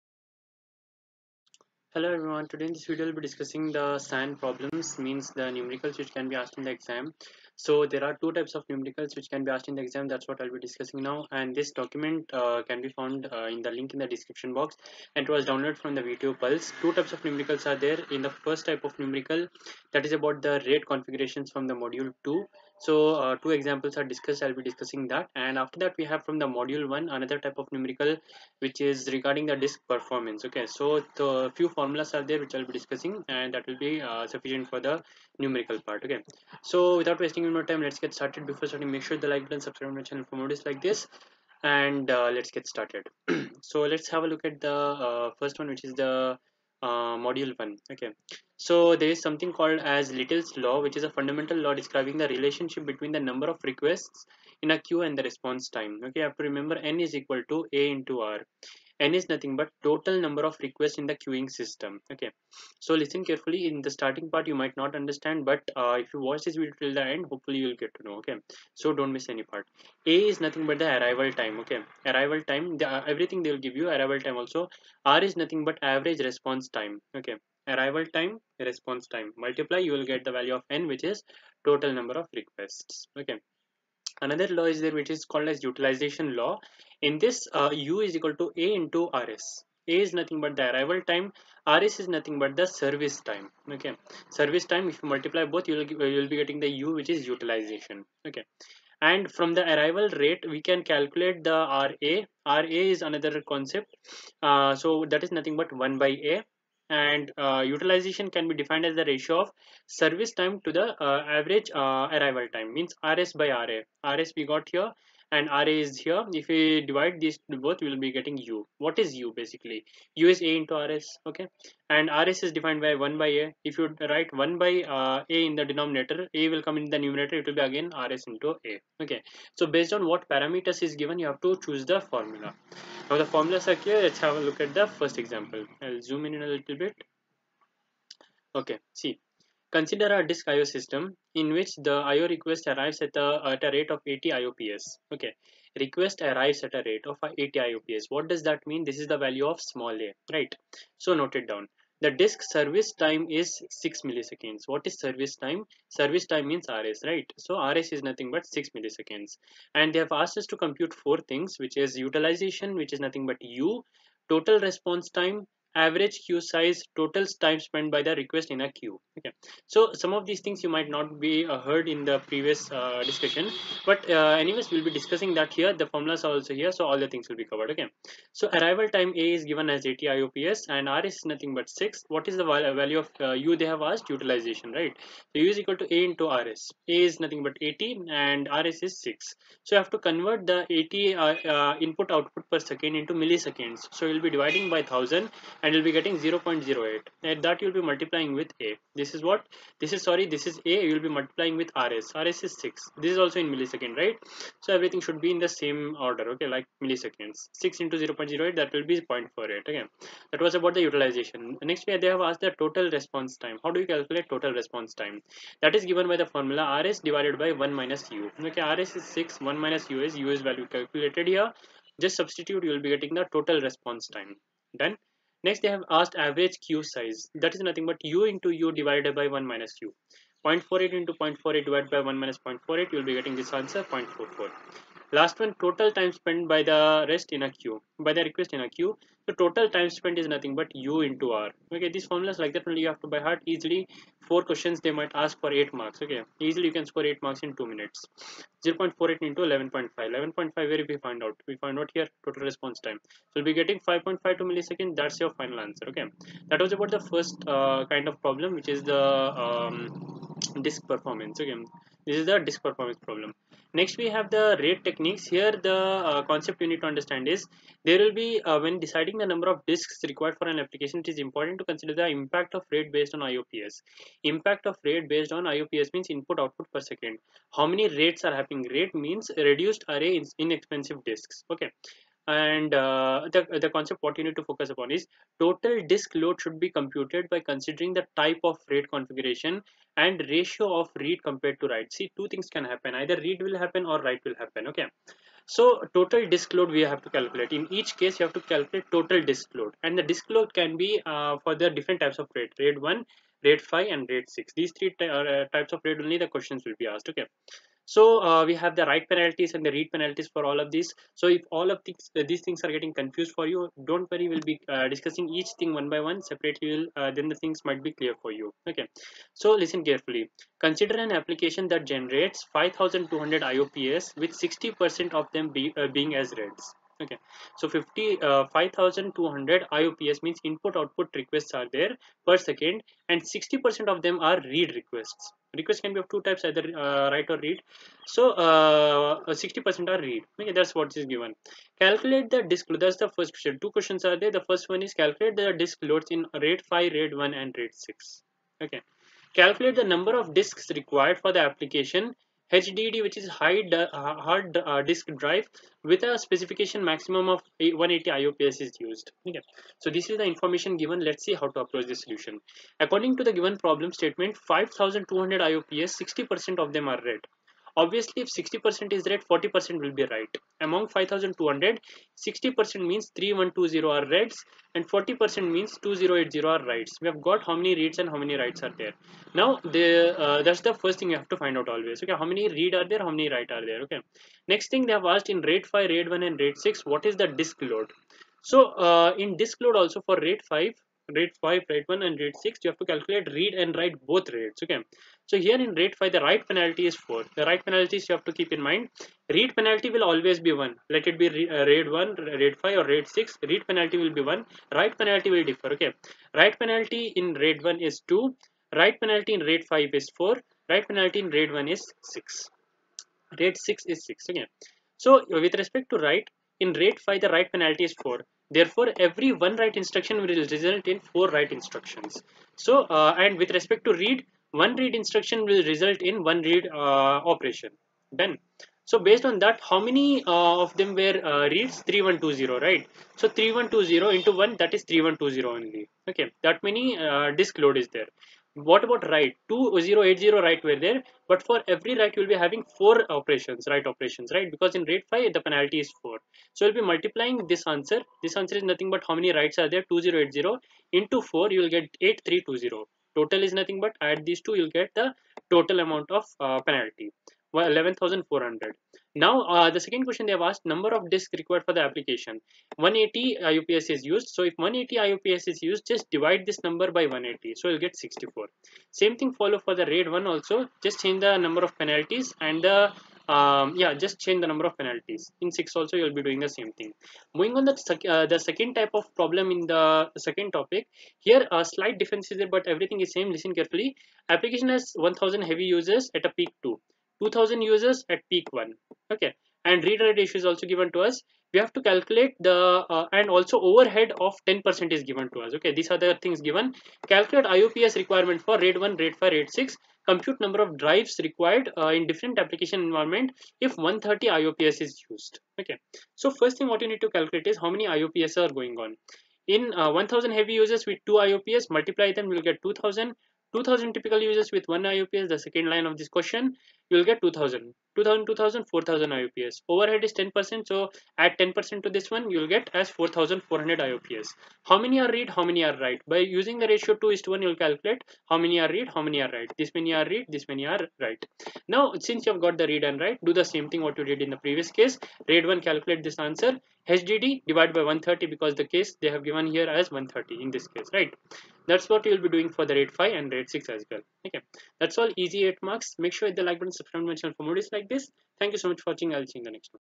SAN 18CS822 EASY NUMERICALS Storage Area Networks [QUICK VIDEO] VTU 8th SEM CSE/ISE
Never Miss the Most Expected Questions from Storage Area Networks . In this video, learn Trends and Patterns seen in the Previous Year VTU exam papers. Selected PYQs 🎯 With Best Shortcuts | Storage Area Networks | Most Expected Questions with Solutions PDF | Afnan Marquee

⌛ Time Stamps

0:00 Disk Performance - Mod 1 🔥🔥🔥
4:24 RAID - Mod 2 🔥🔥🔥